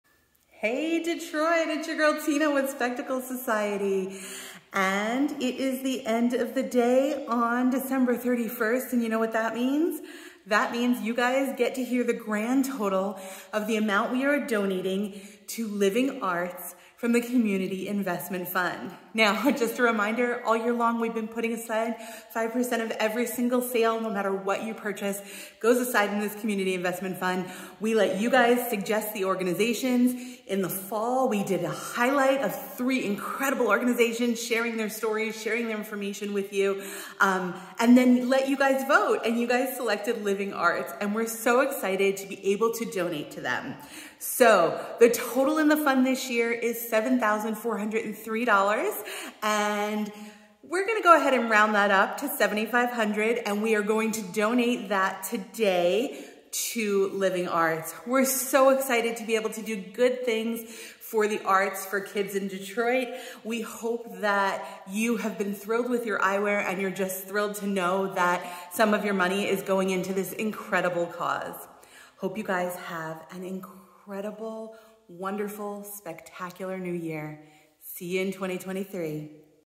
2022 Spectacle Society Community Investment Fund Donation Revealed!!
Another year has come to a close, and that means we have donated our entire Community Investment Fund for 2022 to Living Arts .

Thank you to all the members of our community who have spread the word about our Community Investment Fund, given us non profit suggestions, and who took the time to vote for where you wanted this money to go. We are also grateful to you for amplifying the work that our top three non-profits do for our city.

And a big thank you to all of our clients this year! We are so honored that you welcomed us into your world, shared who you are with us, and allowed us to express that through eyewear. Every pair of glasses, sunglasses and eyewear accessories you purchased helped contribute to this donation. Thank you for choosing to support our small business.

We are thrilled to be able to give this donation to help Living Arts continue their vital work.

Have a fantastic evening tonight Detroit! Happy New Year. We are looking forward to a SPECtacular 2023!